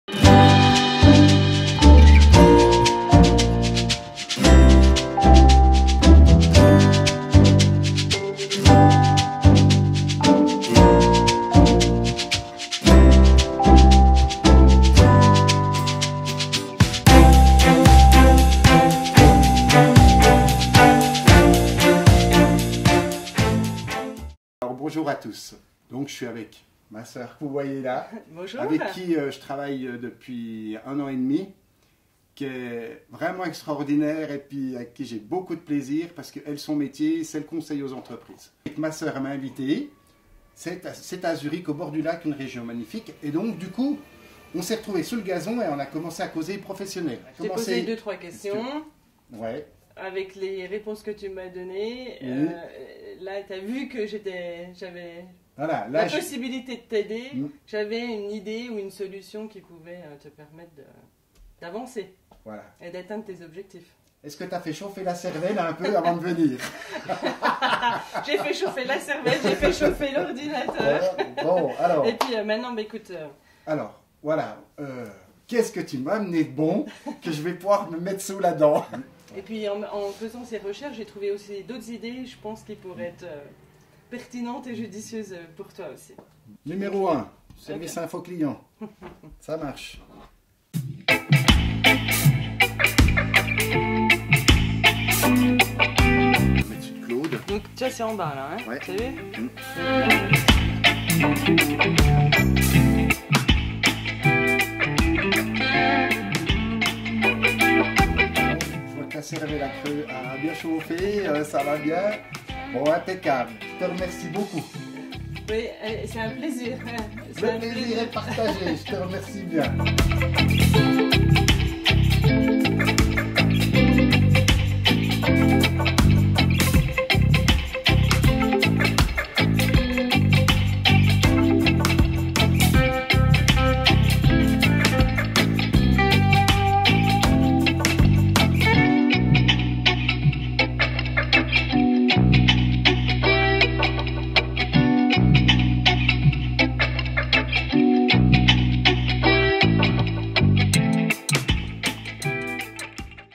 Nouvelles stratégies ou l'art du consulting
Est-ce bien utile de demander une consultation de coaching avec un partenaire d'affaires ? Réponse en images dans une petite entreprise de paysagisme suisse à Vallorbe. Xavier Colisson rencontre une coach en entreprise.